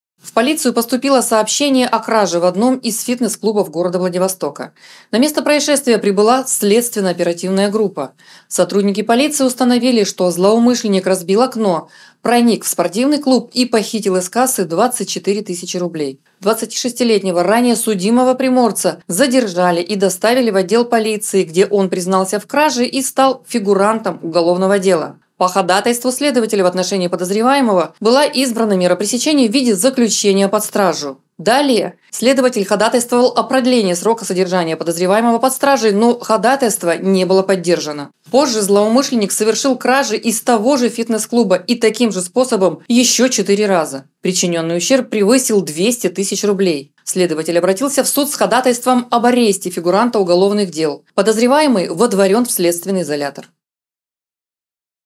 Вора-рецидивиста водворили в СИЗО.
В дежурную часть отдела полиции № 3 (Первореченский район) поступило сообщение о краже от директора одного из фитнес-клубов Владивостока. На место происшествия прибыла следственно-оперативная группа.  Полицейские установили, что злоумышленник, разбив окно, проник в спортивный клуб и похитил из кассы денежные средства в размере свыше 24 тысяч рублей.  Сотрудники полиции оперативно задержали и доставили в отдел полиции 26-летнего ранее судимого жителя Владивостока. Молодой человек дал признательные показания.